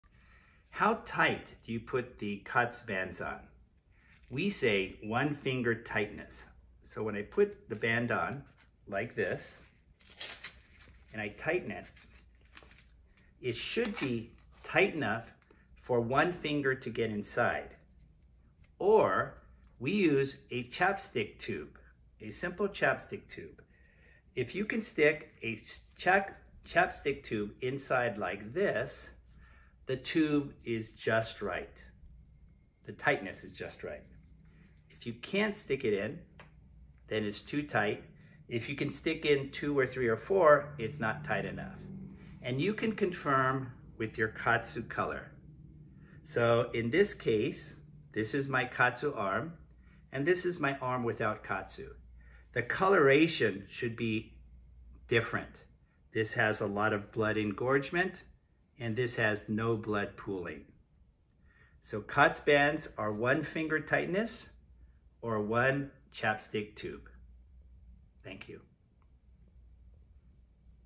Using Chapstick with KAATSU To Measure Appropriate Band Tightness
You can use a Chapstick tube with your KAATSU Air Bands to approximate the appropriate tightness of your Bands.